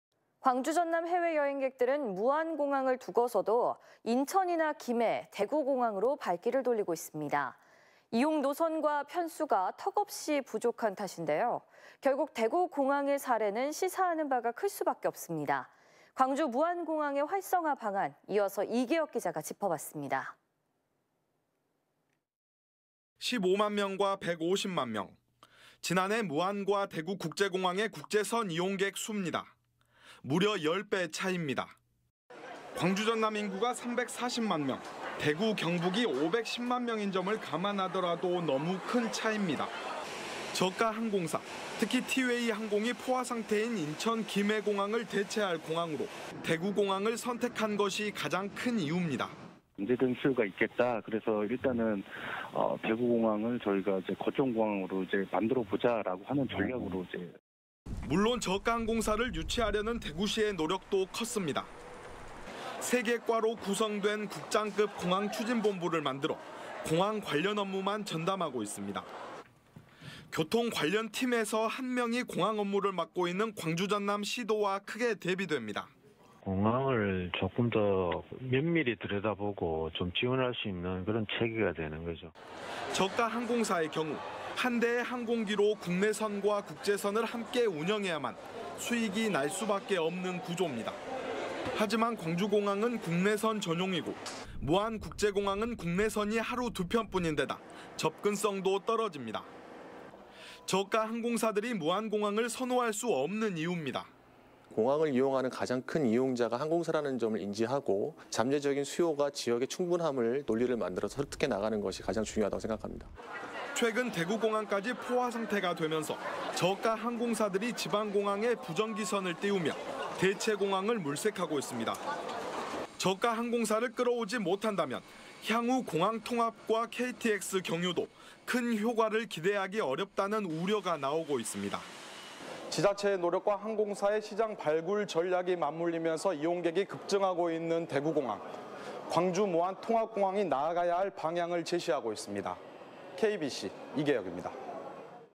"광주ㆍ전남 거점 저가항공사를 찾아라"
광주전남 해외 여행객들은 무안공항을 두고서도 인천이나 김해, 대구공항으로 발길을 돌리고 있습니다.

이용 노선과 편수가 턱없이 부족한 탓인데, 결국 대구공항의 사례는 시사하는 바가 클 수 밖에 없습니다. 광주 무안공항의 활성화 방안, 이어서 이계혁 기자가 짚어봤습니다.